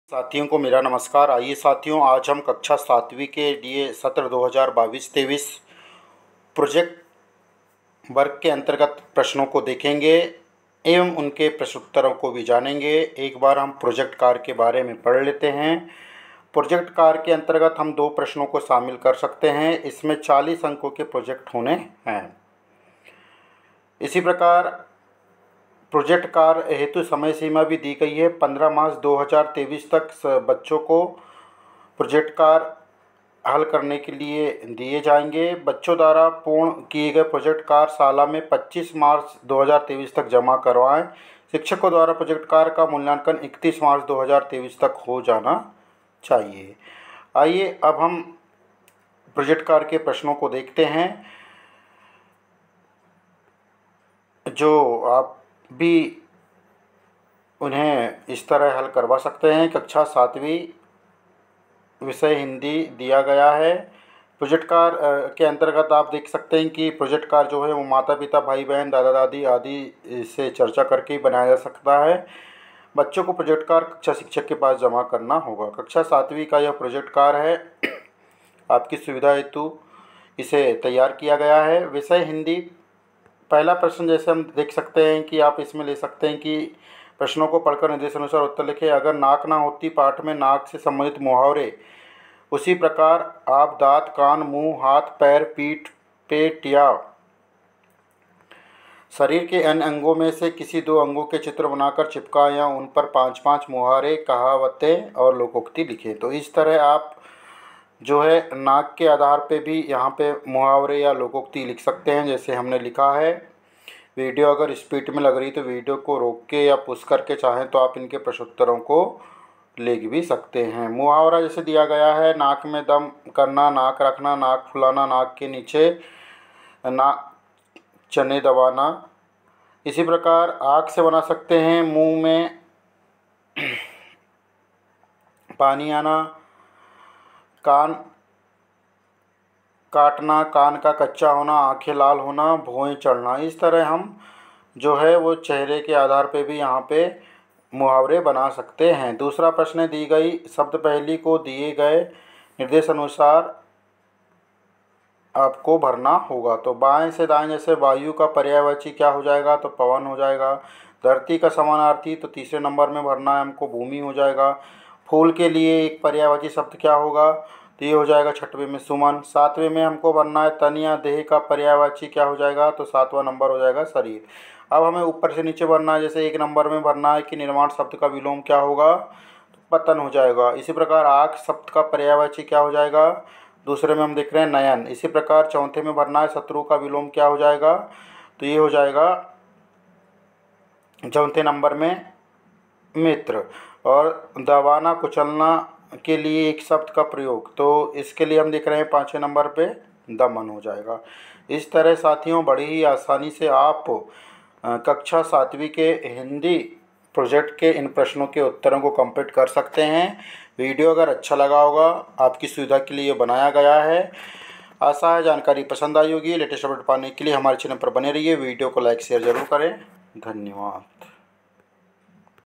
वार्षिक परीक्षा प्रोजेक्ट कार्य 2022-23 प्रश्न उत्तर देखे,कक्षा 7वी,विषय_ हिंदी,पाए 40 अंक ऐसे
RSK के निर्देश अनुसार प्रश्र उत्तर
वार्षिक परीक्षा प्रोजेक्ट कार्य 2022-23 प्रश्न उत्तर देखे,